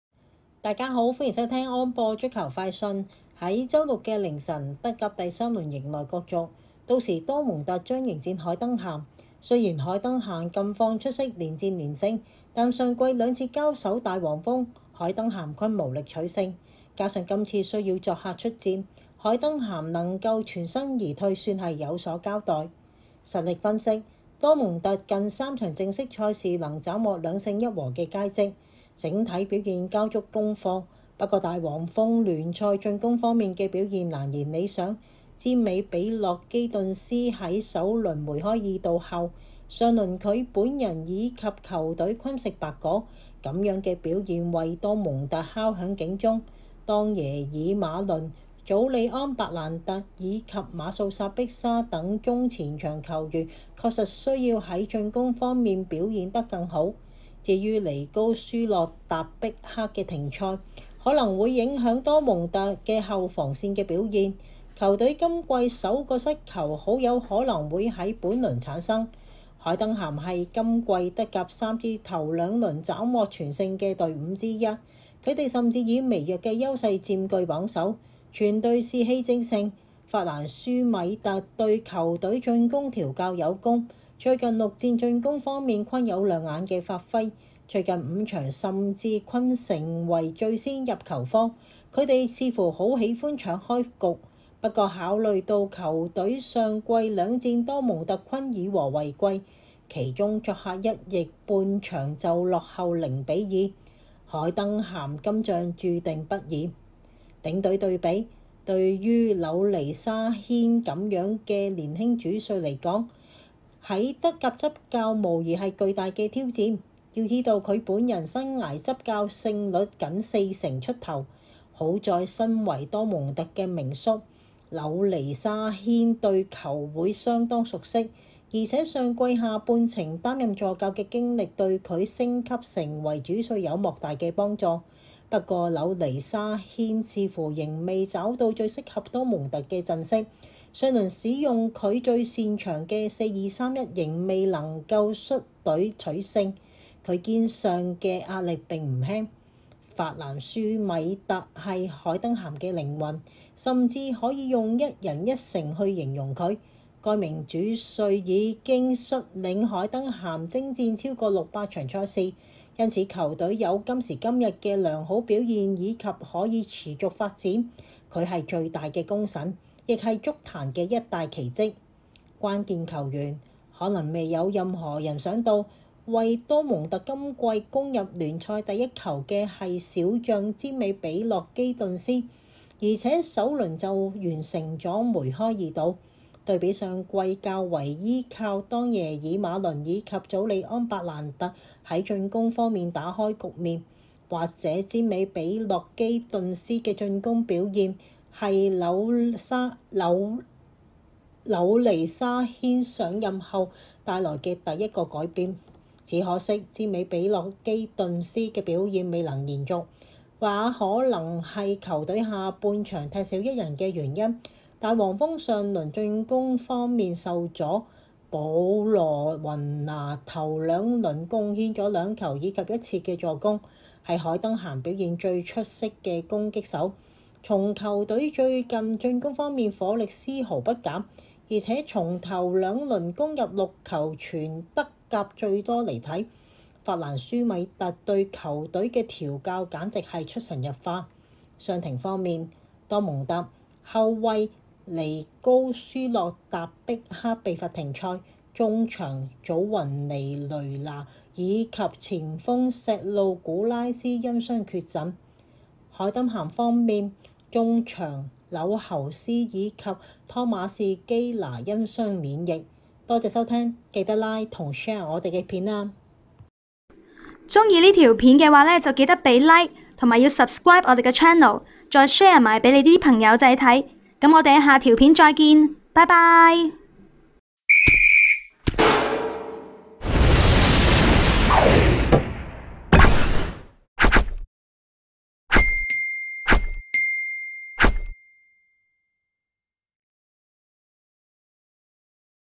【德甲賽前】2024-09-14 多蒙特 VS 海登咸 | 多蒙特恭候海登咸 [聲音報導: Jojo]
週六凌晨，德甲 第三輪迎來角逐，屆時#多蒙特 將主迎#海登咸 。雖然海登咸近況出色連戰連勝，但上季兩次交手大黃蜂，海登咸均無力取勝，加上此番需要作客出戰，海登咸能夠全身而退已算有所交代。

實力分析
多蒙特近3場正式賽事能斬獲2勝1和的佳績，整體表現交足功課。不過大黃蜂聯賽進攻端的表現難言理想，占美.比諾基頓斯在首輪梅開二度後，上輪他本人以及球隊均食白果，這樣的表現為多蒙特敲響警鐘，當耶爾.馬倫、祖利安.白蘭特以及馬素.薩碧沙等中前場球員確實需要在進攻端表現得更好。至於尼高.舒洛達碧克的停賽，可能會影響多蒙特的後防表現，球隊今季首個失球很有可能會在本輪產生。海登咸是今季德甲三支頭2輪斬獲全勝的隊伍之一，他們甚至以微弱優勢佔據榜首，全隊士氣正盛。法蘭舒米特對球隊進攻調教有功，最近6戰進攻端均有亮眼發揮，最近5場甚至均成為最先入球方，他們似乎很喜歡搶開局，不過考慮到球隊上季兩戰多蒙特均以和為貴，其中作客一役半場就落後0比2，海登咸此行注定不易。

領隊對比
對於紐尼沙軒這樣的年輕主帥來說，在德甲執教無疑是巨大挑戰，要知道他本人生涯執教勝率僅四成出頭。好在身為多蒙特名宿，紐尼沙軒對球會相當熟悉，而且上季下半程擔任助教的經歷對他升級成為主帥有莫大幫助。不過紐尼沙軒似乎仍未找到最適合多蒙特的陣式，上輪使用他最擅長的4-2-3-1亦未能率隊取勝，他肩上的壓力並不輕。法蘭舒米特是海登咸的靈魂，甚至可以用一人一城去形容他，該名主帥已經率領海登咸征戰超過600場賽事，因此球隊有今時今日的良好表現以及可持續性發展，他是最大功臣，亦是足壇的一大奇蹟。

關鍵球員
可能沒有任何人想到，為多蒙特今季攻入聯賽第一球的是小將占美.比諾基頓斯，而且首輪就完成了梅開二度，對比上季較為依靠當耶爾.馬倫以及祖利安.白蘭特在進攻端打開局面，或者占美.比諾基頓斯的進攻表現，是紐尼沙軒上任後帶來的第一個改變。只可惜占美.比諾基頓斯的表現未能延續，也可能是球隊下半場踢少一人的原因，大黃蜂上輪進攻端受阻。保羅.雲拿頭2輪貢獻2球以及1次助攻，是海登咸表現最出色的攻擊手，從球隊最近進攻端火力絲毫不減，而且從頭2輪攻入6球全德甲最多來看，法蘭舒米特對球隊的調教簡直出神入化。

傷停情況
多蒙特：後衛尼高.舒洛達碧克被罰停賽；中場祖雲尼.雷拿因傷缺席。
海登咸：中場紐侯斯以及湯馬士基拿因傷免役。